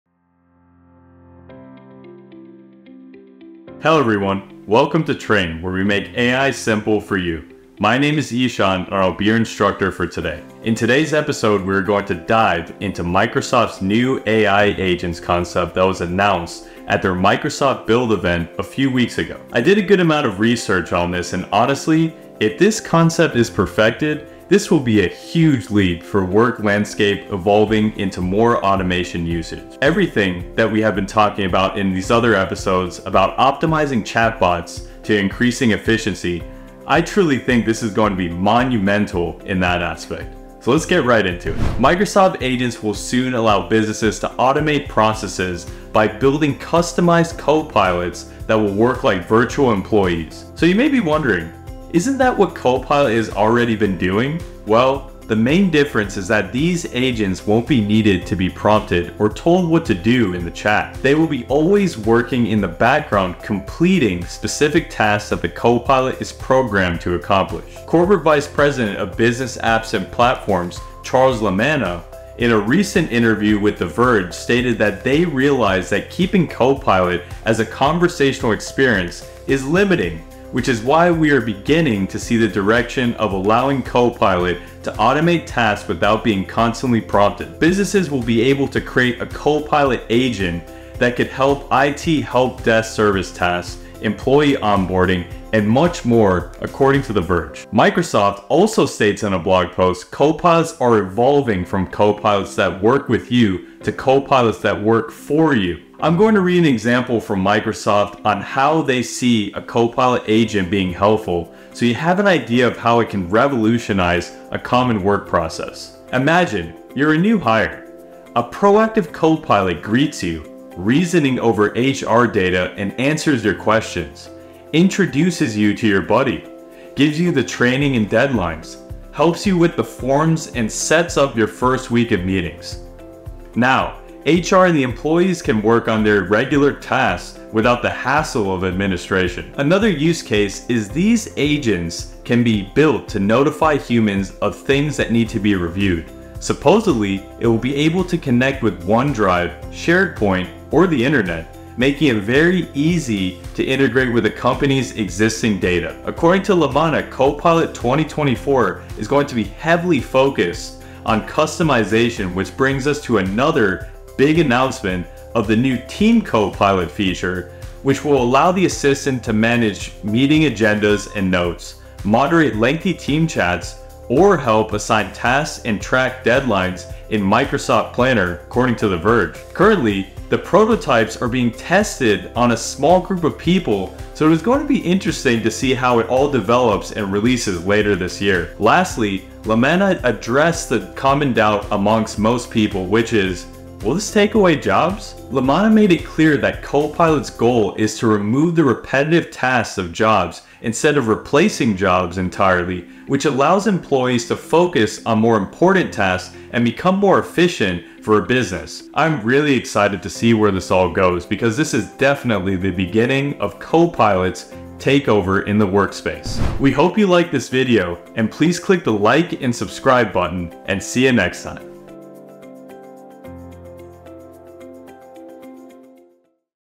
How Microsoft Copilot Just Changed The AI World FOREVER 🤯📈
Welcome back to trAin! In this video, we dive deep into Microsoft's groundbreaking announcement at Microsoft Build 2024: Copilot AI Agents and Team Copilot. Join us as we explore how these advanced AI tools are set to transform productivity, collaboration, and innovation across various industries.

Whether you're an AI enthusiast, a tech professional, or just curious about the latest advancements, this video will provide you with all the insights you need to stay ahead of the curve.

Chapters:
0:00-0:41 AI Agents Introduction
0:42-1:45 New Features
1:46-2:43 Use Cases
2:44-3:09 Team Copilot Feature
3:10- 3:29 Will this take away jobs?
3:30-3:47 Closing Remarks